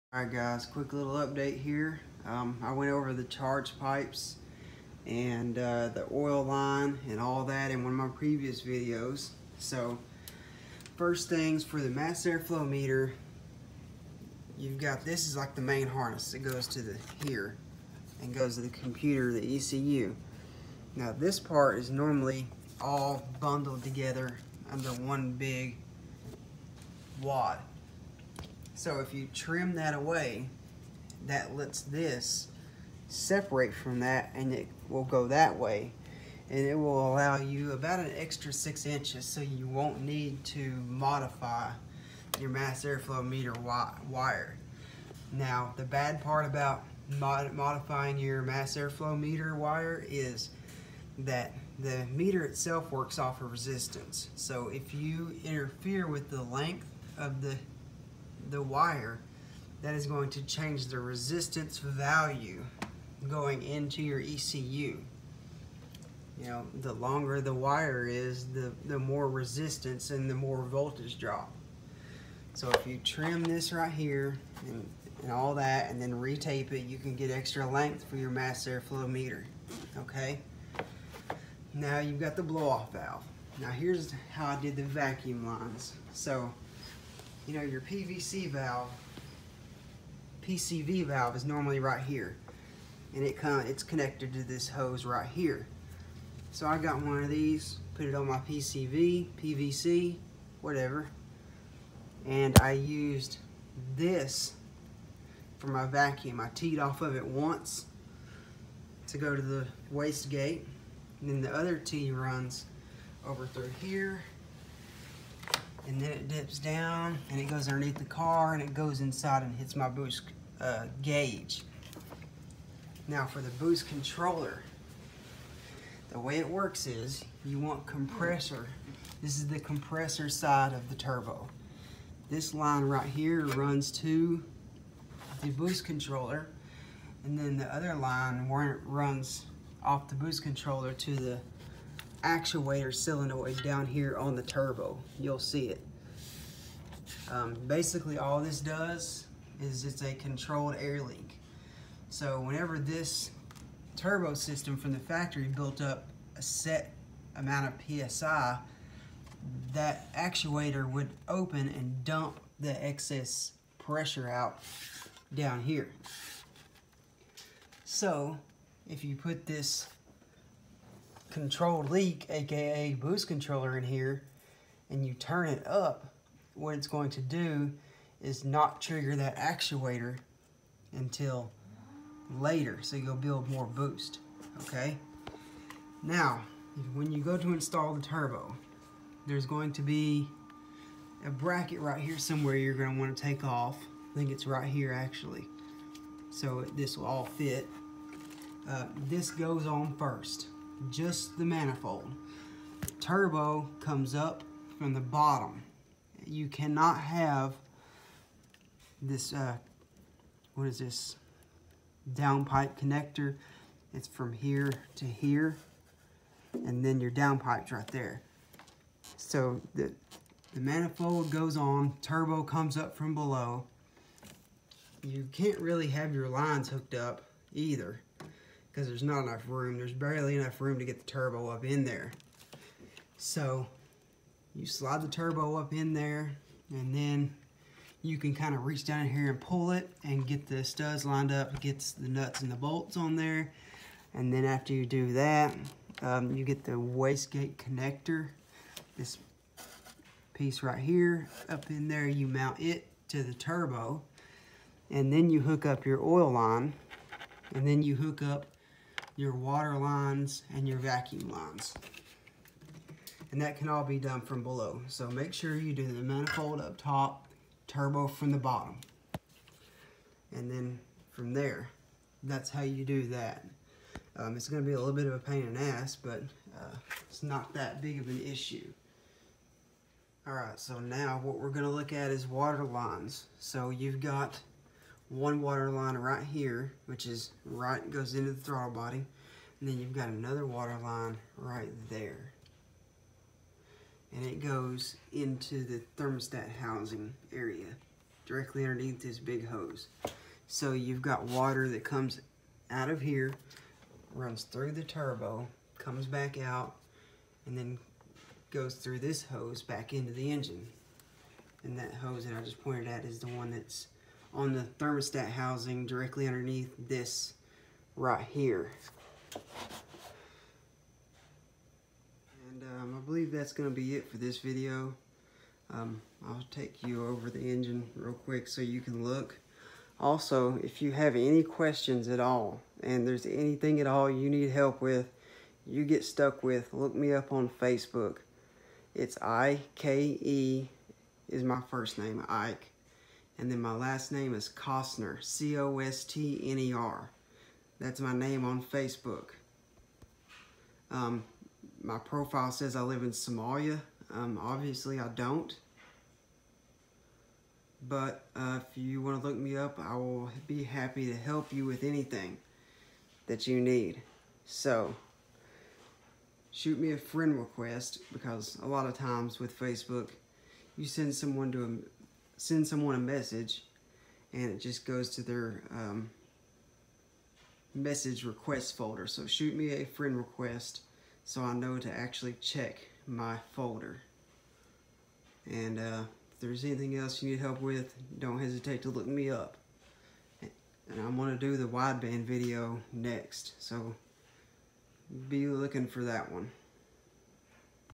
How to Turbo an ES Lancer ep.4 turbo install, water lines, manifold, mass airflow meter & vacuum
Episode 4 hits on turbo manifold, turbo, water lines, vacuum lines, mass airflow meter problems